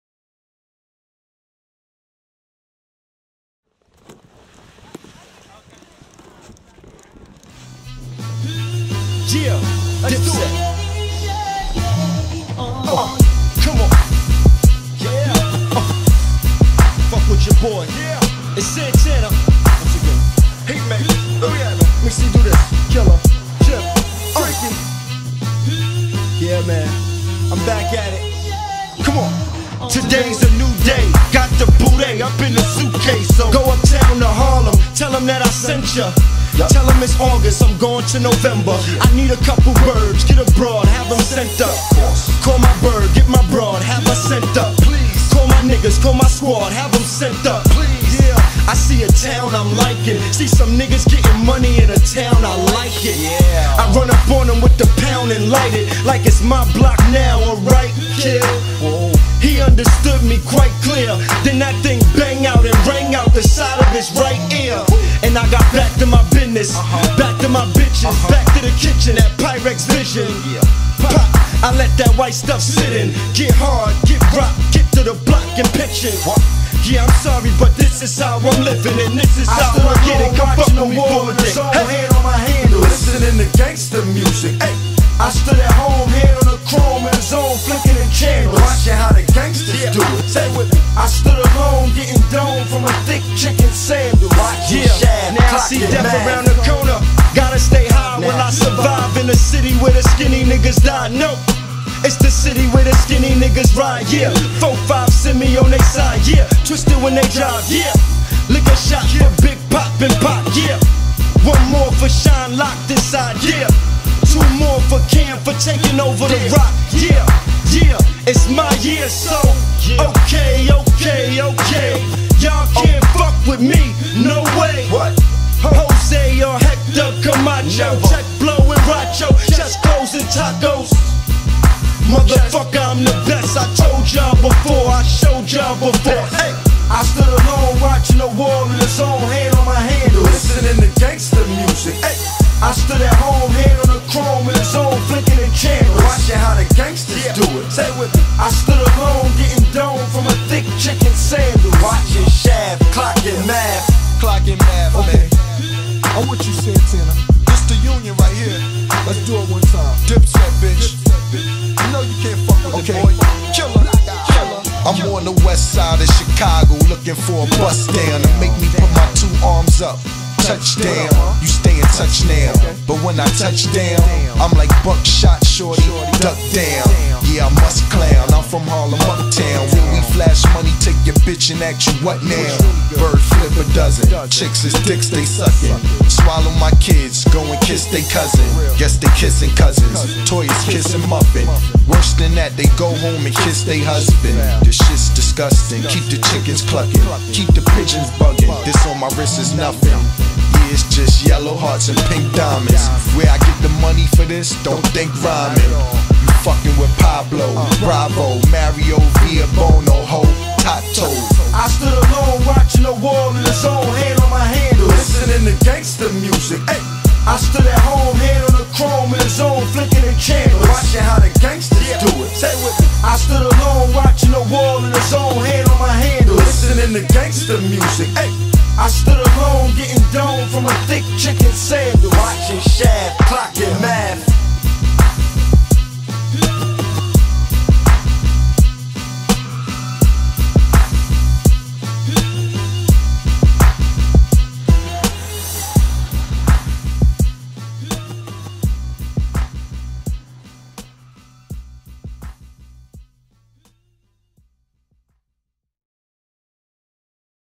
Adam Giola
Adam Giola snowboarding. He rides for Skate Hungry. Filmed at Pine Knob by Will Gencay.